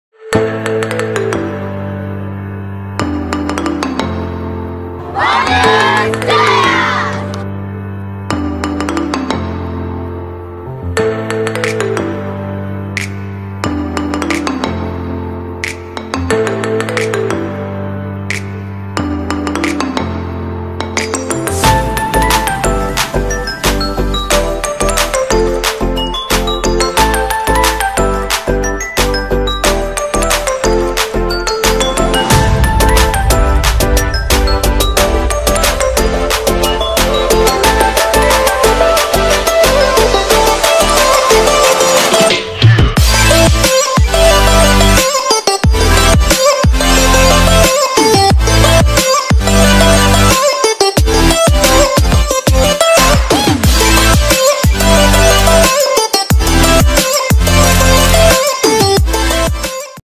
Class Metting 2K18 - SMA Giki 2 Surabaya
SMA GIKI 2 SURABAYA, mengadakan acara class meeting dimana berapa kelas saling bertemu dan berlomba-lomba untuk meraih juara. Apasaja lombanya? bagaimana keseruannya? langsung saja lihat videonya.
THANKS TO
- Allah SWT
- SMA Giki 2 Surabaya
- OSIS SMA Giki 2 Surabaya